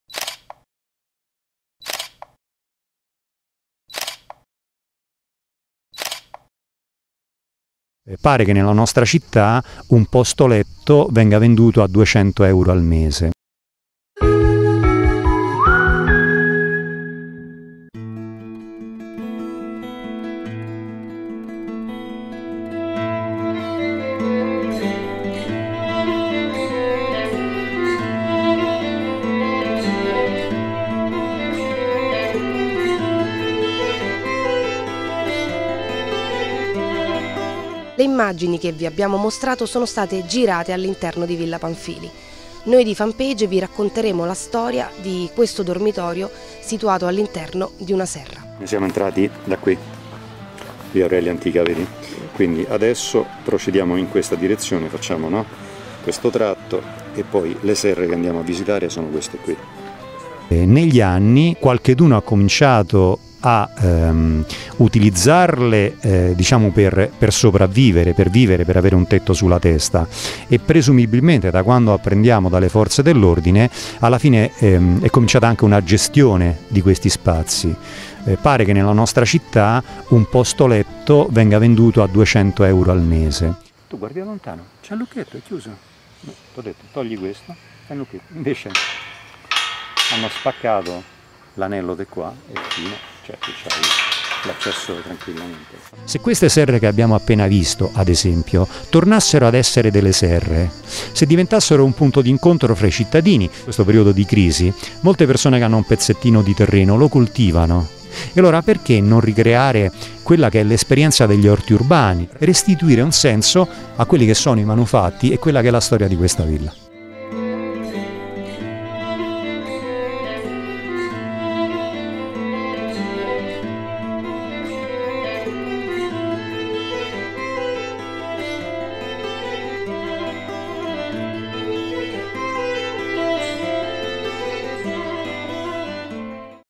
Villa Pamphilj, la serra trasformata in dormitorio: un posto letto costa 200 euro al mese
Materassi, coperte e oggetti personali ma anche preservativi e sporcizia. Nelle serre di Villa Pamphilj, nascoste tra il verde, c'è un vero e proprio dormitorio abusivo. Tante le segnalazioni dei cittadini per sgomberare l'area, ma i posti letto sono ancora lì. Probabilmente non si tratta di singoli senzatetto, ma c'è una vera e propria organizzazione dei materassi: un posto letto nella serra di Villa Pamphilj costa 200 euro al mese.